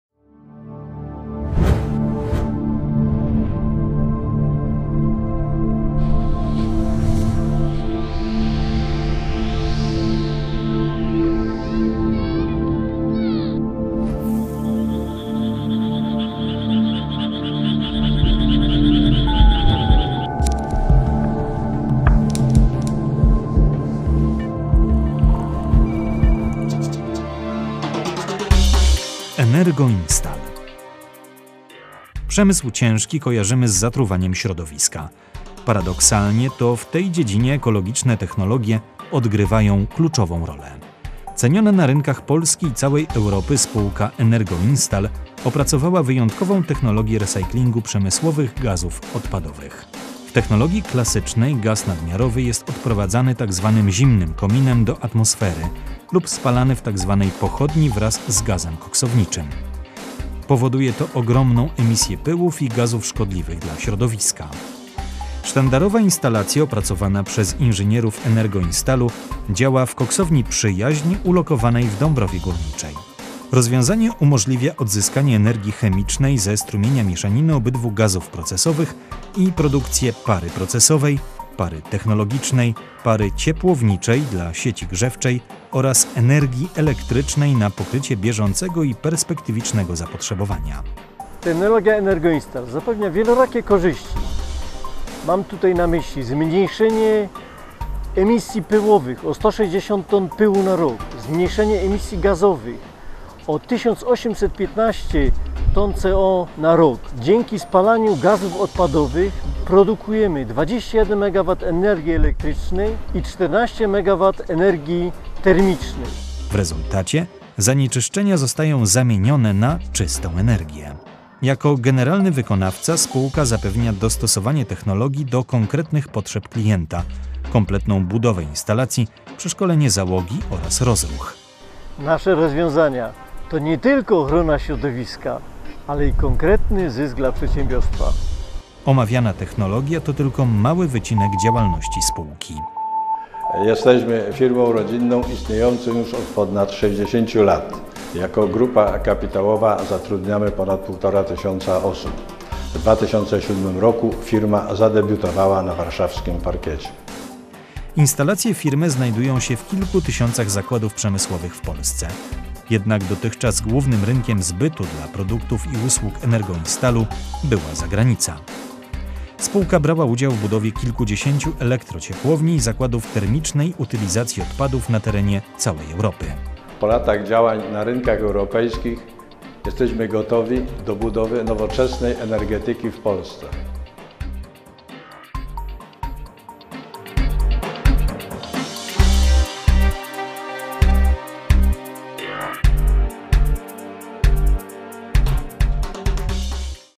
GreenEvo: ENERGOINSTAL
ENERGOINSTAL SA  to jeden z największych producentów kotłów energetycznych w Polsce, znany i ceniony partner na rynku krajowym i zagranicznym. Swoją pozycję przedsiębiorstwo zawdzięcza stałemu unowocześnianiu technologii wytwarzania oraz elastycznemu reagowaniu na zmieniające się oczekiwania rynku.

ENERGOINSTAL SA może poszczycić się pozycją jedynego w Polsce i jednego z pięciu w Europie producenta rur ożebrowanych - podstawowego elementu kotłów odzysknicowych i wymienników ciepła, w tym z materiału P91 na parametry nadkrytyczne. Rury te znalazły zastosowanie w wybudowanych przez ENERGOINSTAL SA dotychczas największych kotłach odzysknicowych w Europie (Dunkierka, Francja). Innym elementem ciśnieniowym kotłów o podobnych walorach jak powyższe są produkowane przez firmę ściany membranowe. Unikatowa technologia produkcji pozwala na wysoką jakość produktu, atrakcyjne ceny oraz zapewnia nabywców na rynkach europejskich i światowych.

Produkcja: Servimedia